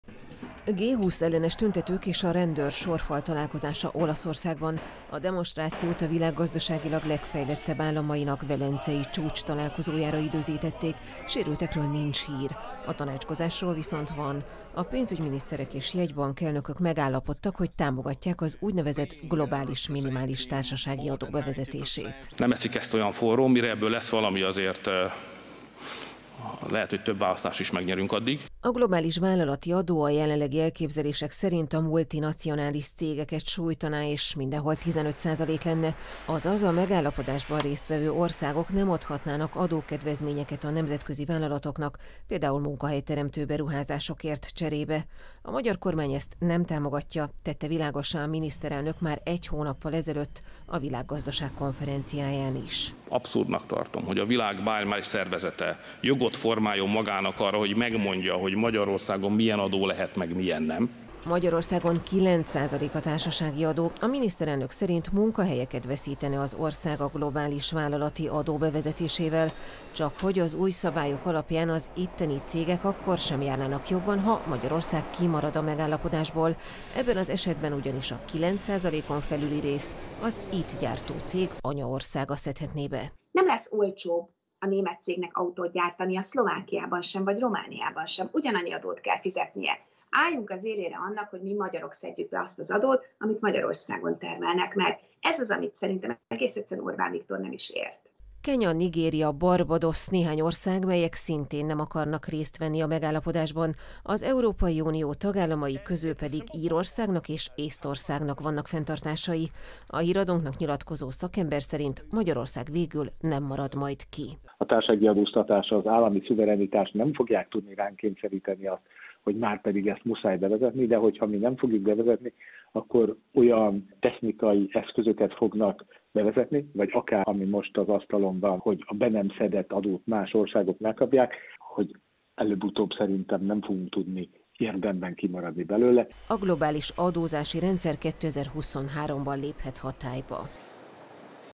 A magyar kormány nem támogatja a globális minimumadó bevezetését 21-07-11
A világ több mint 130 országa támogatja a globális minimumadó bevezetését, a magyar kormány viszont abszurdnak tartja az ötletet. Szerintük azzal munkahelyeket veszítene az ország. A DK-nál azt mondják, ez teljesen téves elképzelés. A Híradónknak nyilatkozó szakértő pedig arra számít, hogy a megállapodásból végül Magyarország sem marad ki.